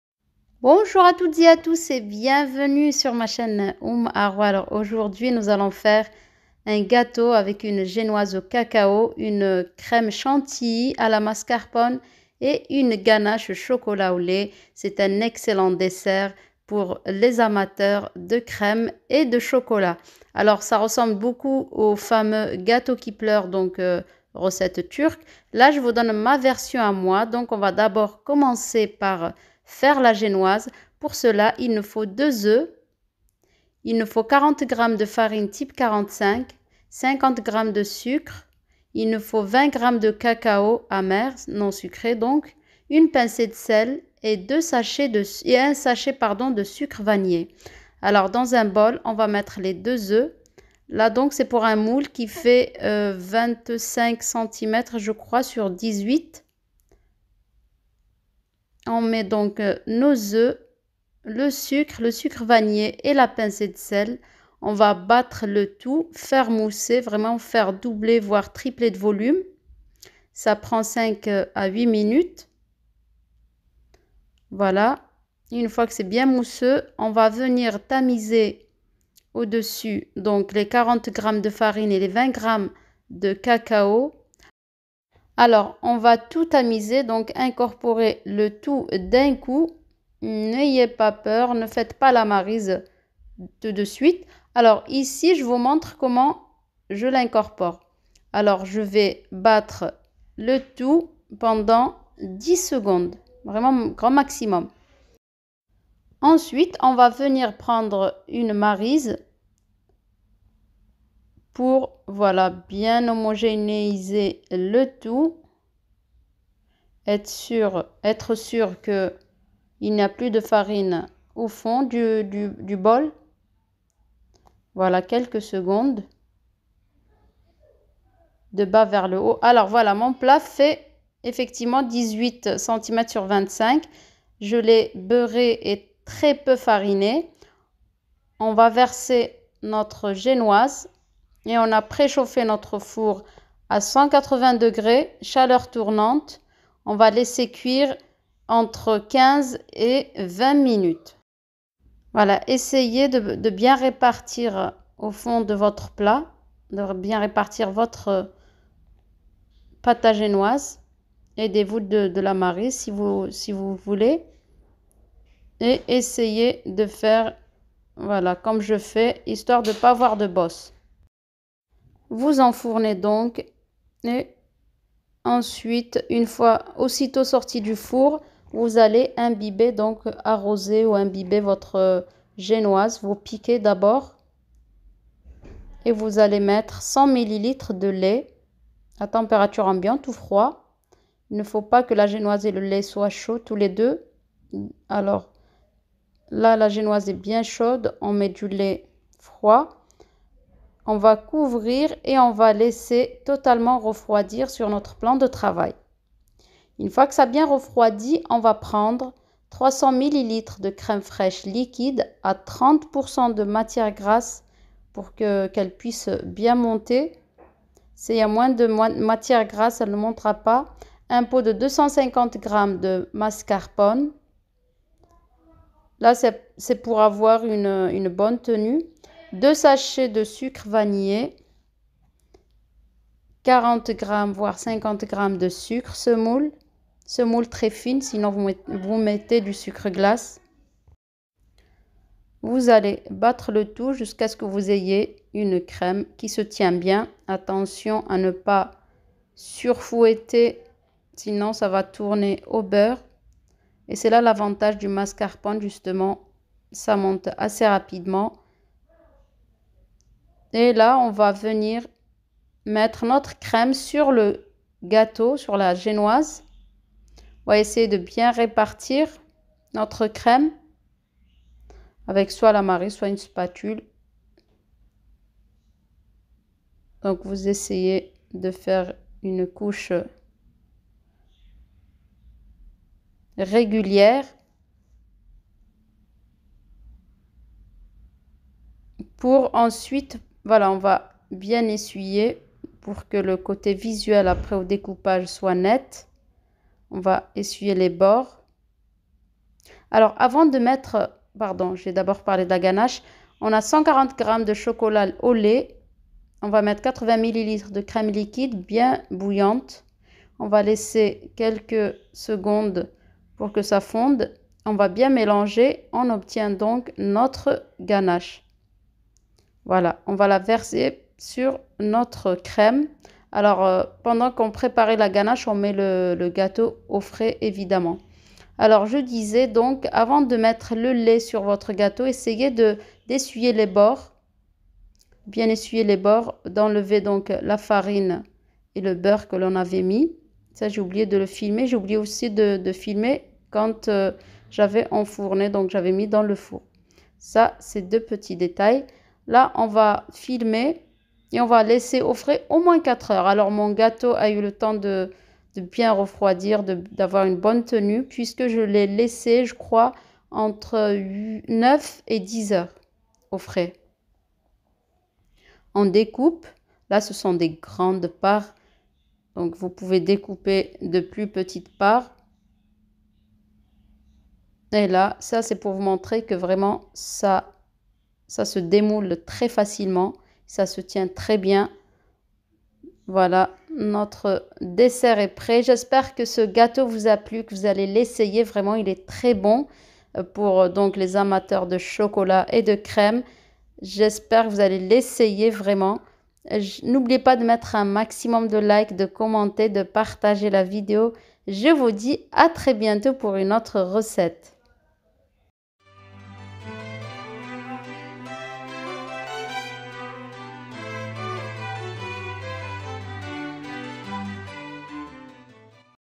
le célèbre gâteau qui pleure à ma façon ,idée dessert
Aujourd'hui on va faire un gâteau au chocolat à la crème chantilly...
ingrédients:
*génoise:
2 oeufs
50g sucre
pincée de sel
40g farine
20g cacao non sucré
1 sachet sucre vanillé
*crème:
300ml crème liquide à 30%m.g.
50g sucre
2 sachets sucre vanillé
250g mascarpone
*gânache:
140g chocolat au lait
80ml crème liquide

Hello everyone!!!
Today we are going to make a chocolate cake with whipped cream...
ingredients:
*Genoese:
2 eggs
50g sugar
pinch of salt
40g flour
20g unsweetened cocoa
1 sachet of vanilla sugar
*cream:
300ml liquid cream at 30%m.g.
50g sugar
2 sachets of vanilla sugar
250g mascarpone
*ganache:
140g milk chocolate
 80ml liquid cream

صباح الخير جميعا!!! اليوم سنقوم بتحضير كعكة الشوكولاتة بالكريمة المخفوقة.
مكونات:
*الجنوة:
2 بيضة
50 جرام سكر
قليل من الملح
40 جرام دقيق
20 جرام كاكاو غير محلى
1 كيس سكر فانيليا
*كريم:
300 مل كريم سائل بتركيز 30% م.د.
50 جرام سكر
2 كيس سكر فانيليا
250 جرام ماسكربون
*الغاناش:
140 جرام شوكولاتة بالحليب
80 مل كريمة سائلة

¡¡¡Buenos días a todos!!! Hoy vamos a hacer una tarta de chocolate con nata montada... ingredientes:
*genovés:
2 huevos
50 g de azúcar
pizca de sal
40 g de harina
20 g de cacao sin azúcar
1 sobre de azúcar de vainilla
*crema:
300ml de nata líquida al 30%m.g.
50 g de azúcar
2 sobres de azúcar de vainilla
250 g de mascarpone
*ganache:
140 g de chocolate con leche
80ml de nata líquida